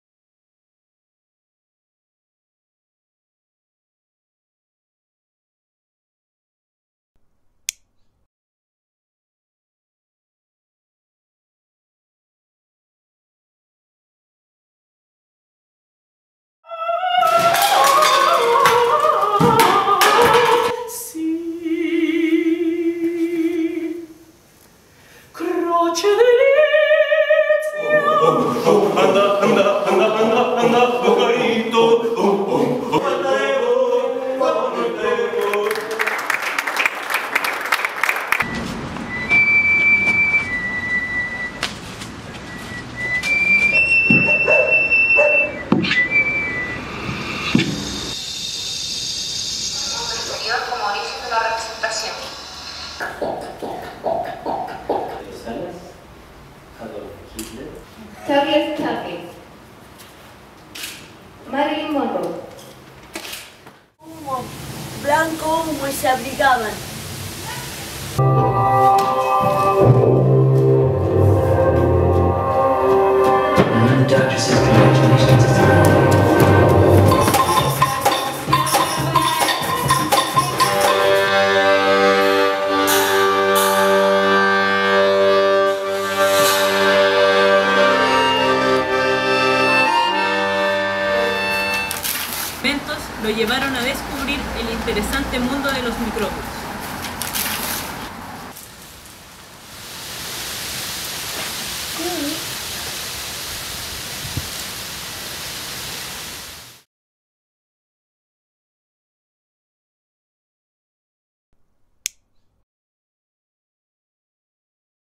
2º Encuentro Escenas al SXXI - Cuerpo y Memoria
2º Encuentro Escenas al SXXI Cuerpo y Memoria
1ª Semana
- PAMPA Carolina Cifras
- COMO UN PERRO AL SOL Performer Persona Project
- TRAFKINTUN Territorio Kimun
- ACCIONES PARA RECORDAR Danza La Independiente
- SHOCK Compañia Teatro Del Cuerpo
- RECURRENTE TEMBLOR Colectivo Danza Asac
- UNA ESCOLAR LINEA DE TIEMPO Paula Baeza